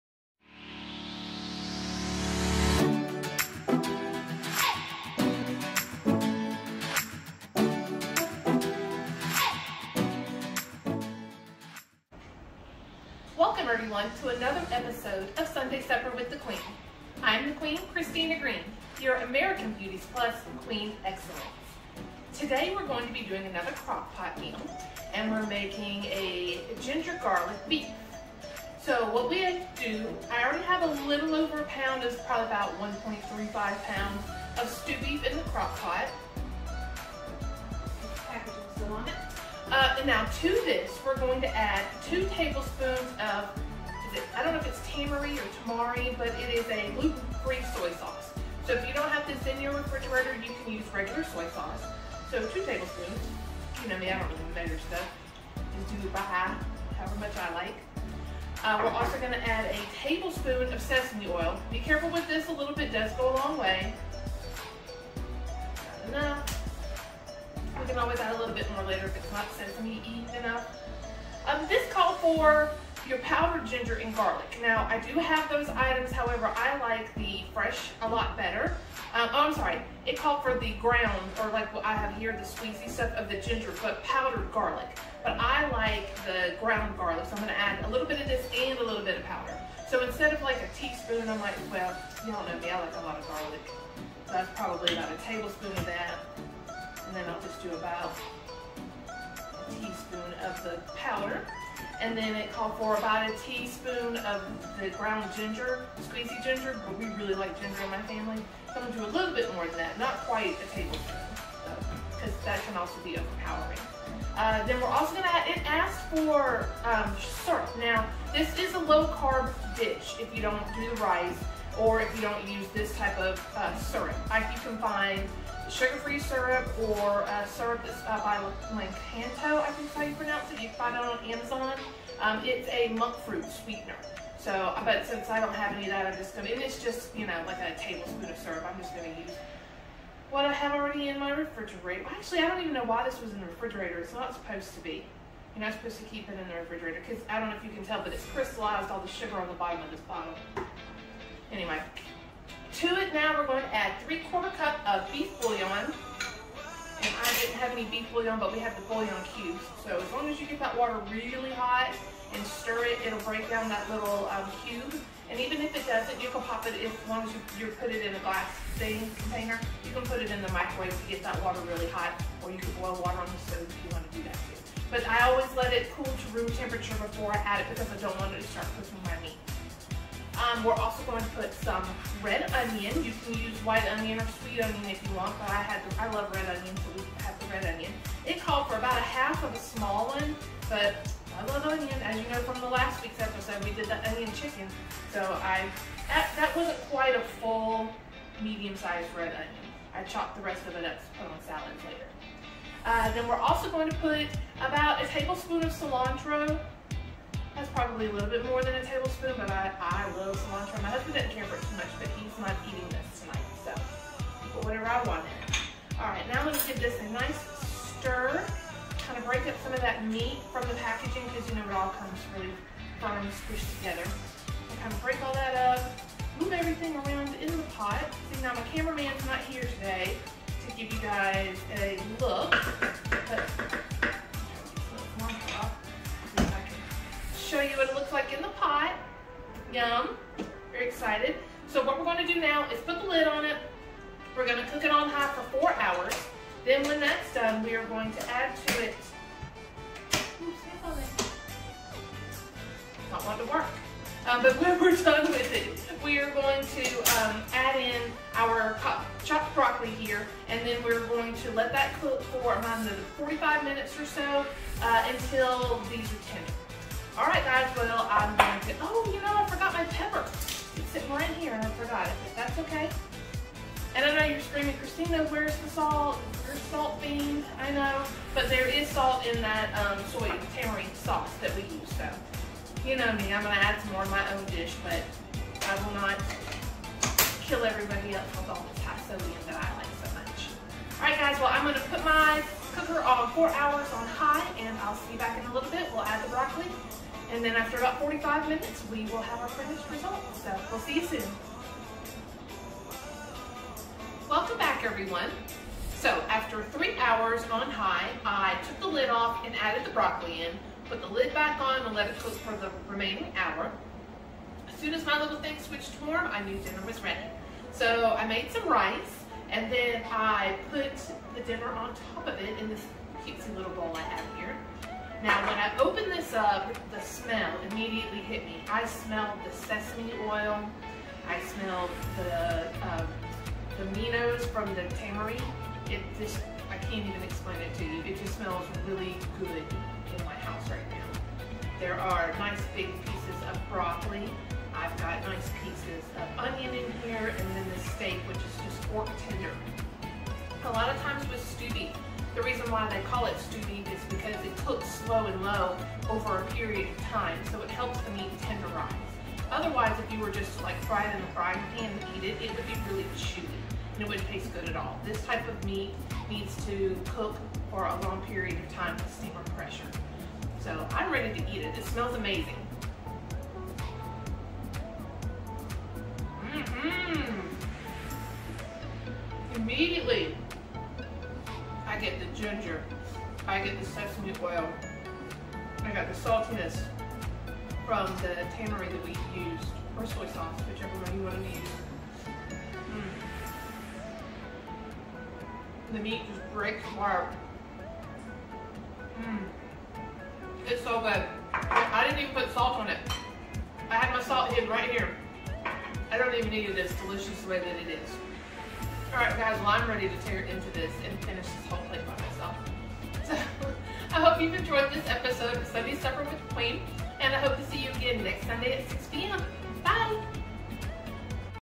Garlic Ginger Beef
It's time for another episode of Sunday Supper with the Queen! Tonight we are having Ginger Garlic Beef. Yummy!

Ingredients:
1# Cubed Beef
2T Tamari
1T Sesame Oil
1t Ground Ginger
1t Garlic Powder
1 cup Broccoli Florets
1 T Maple Syrup
3/4 c Beef Broth
1/2 Onion, sliced
Salt & Pepper
1T Cilantro

Instructions:
Add diced beef to crockpot. Add ginger, garlic, salt and pepper, syrup, oil, and tamari sauce. Mic well. Add stock, onion, and cilantro. Cover and cook on high for 3 hours. Add broccoli, cover, and cook for an additional 45 minutes or until broccoli is tender. Enjoy!